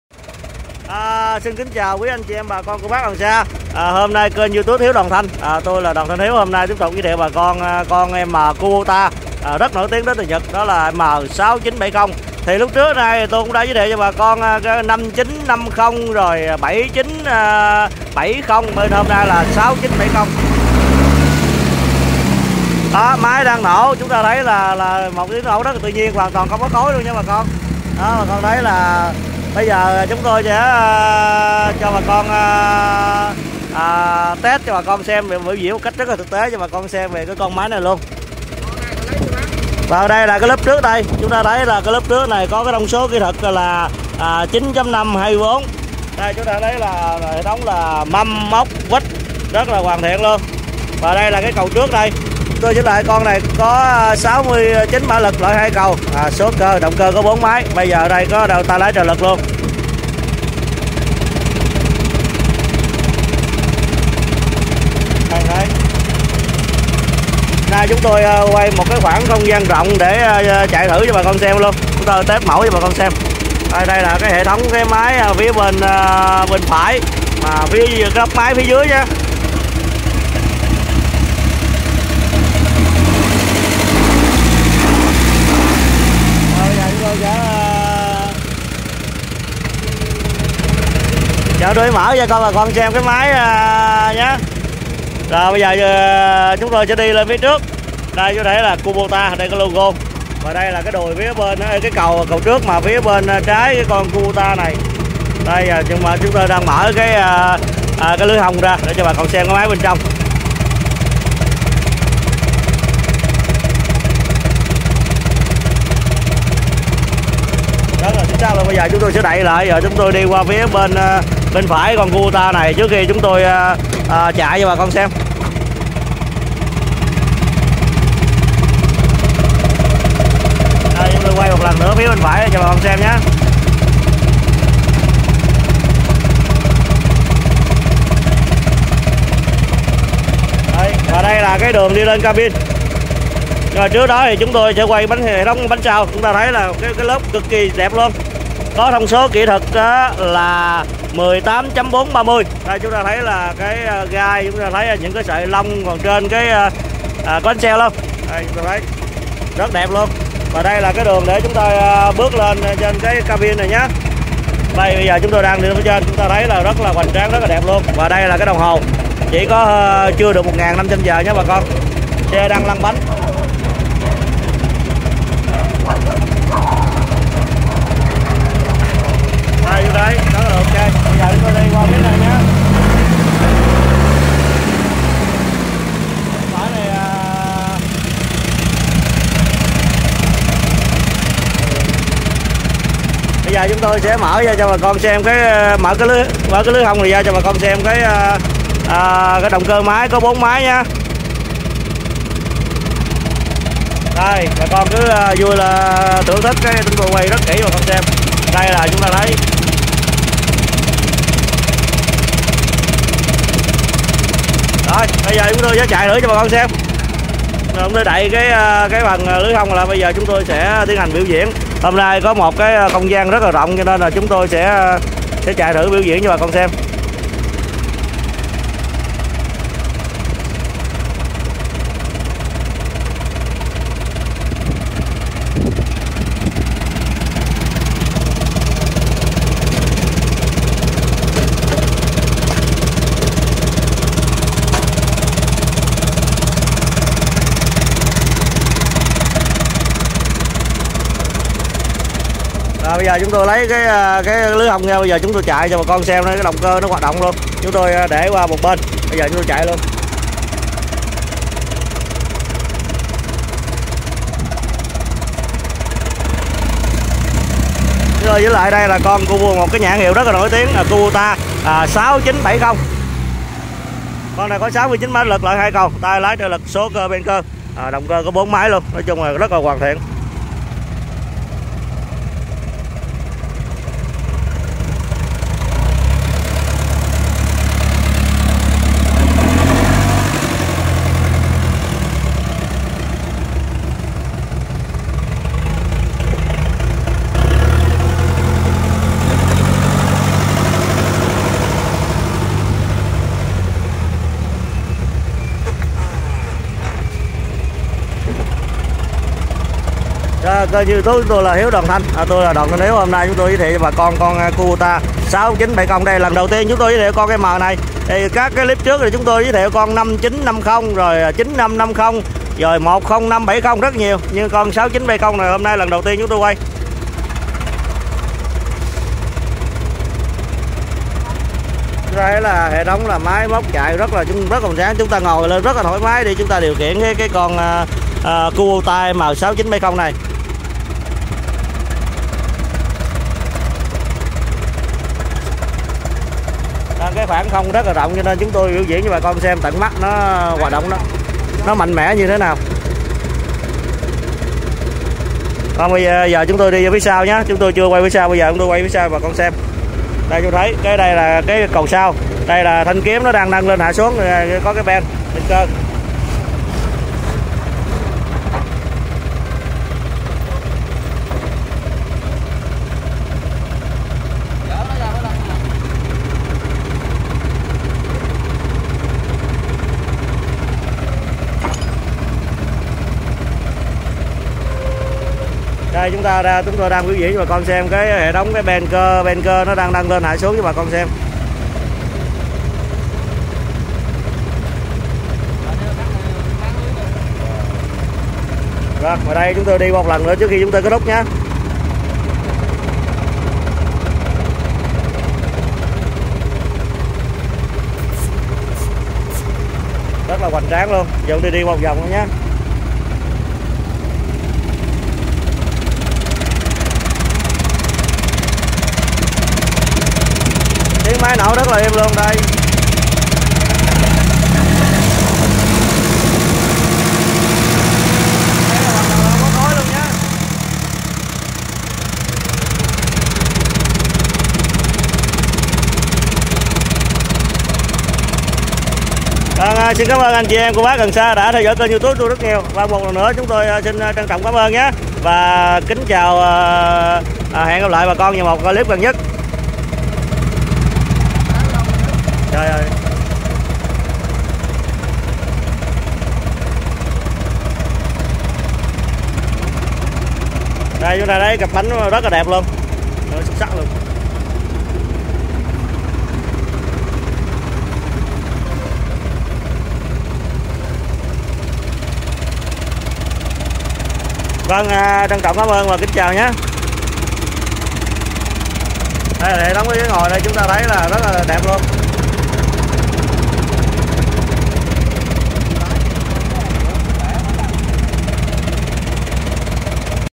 Máy cày KUBOTA M6970 rất tuyệt hàng Nhật,LH:0909389726[Hiếu Đoàn Thanh]
Kubota M6970 với số cơ ben cơ, tay lái trợ lực, động cơ 4 máy rất tuyệt vời cho mọi người